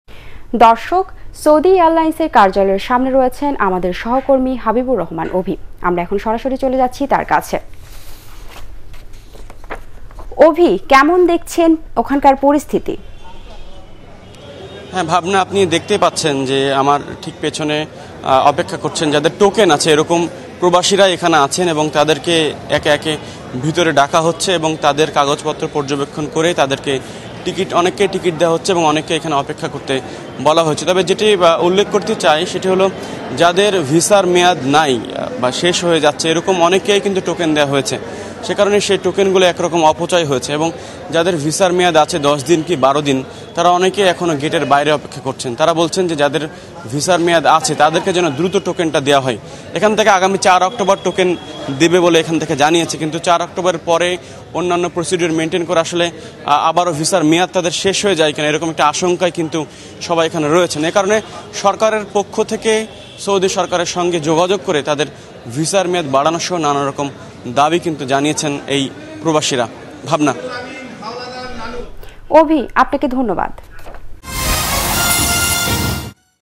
রাজধানীর কারওয়ান বাজারে ভোর থেকেই সৌদি এয়ারলাইনসের কার্যালয়ের সামনে ভিড় করছেন টিকিট প্রত্যাশীরা।
রাজধানীর কারওয়ান বাজারে ভোর থেকেই সৌদি এয়ারলাইনসের কার্যালয়ের সামনে ভিড় করছেন টিকিট প্রত্যাশীরা। ২ হাজার ৭০১ থেকে ৩ হাজার সিরিয়াল পর্যন্ত মোট ৩০০ টোকেনধারীকে টিকিট দেয়া হবে আজ। তবে এর বাইরেও কয়েকশ’ প্রবাসী জড়ো হয়েছেন সেখানে। তাদের অনেকেরই ভিসার মেয়াদ শেষ হয়ে যাবে আজ। তাই ভিসার মেয়াদ বাড়ানোসহ নানা দাবি জানিয়েছেন তাঁরা।